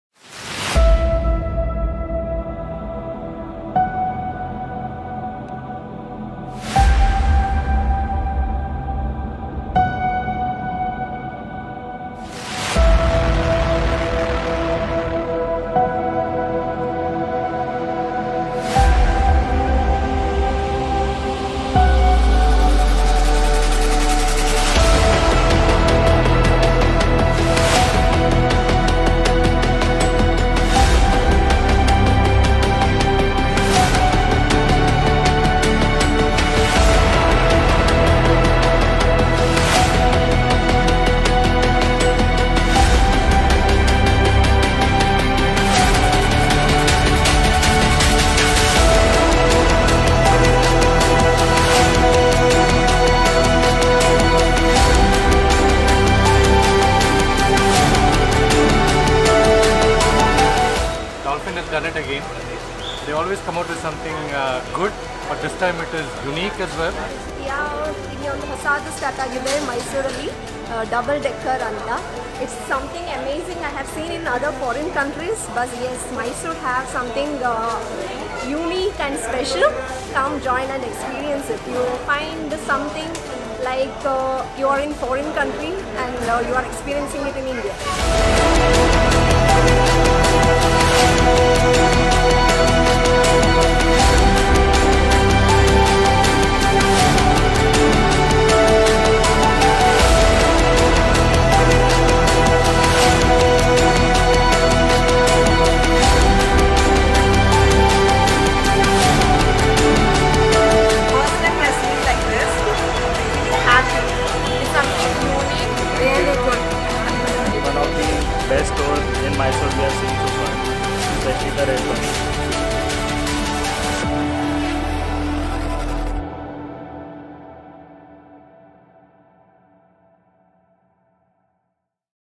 Dolphin Bakes ‘N’ Ice Cream | Mysore | Shaheer Ahmed
Dolphin a popular bakery chain of Mysuru with 13 outlets and it has a new concept- bakery in Double Decker bus with a proud statement “I Love Mysuru Dolphin”